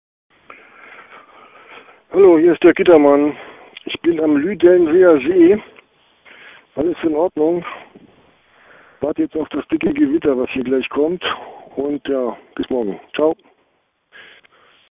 Hans-Dirk G. - Grenzsteintrophy 19.06.13 - 21:30
This vid is part of our riders documentation. Every rider is asked to leave a daily note in our voice mail. Well post it here with a picture of the rider at the starting line. Further info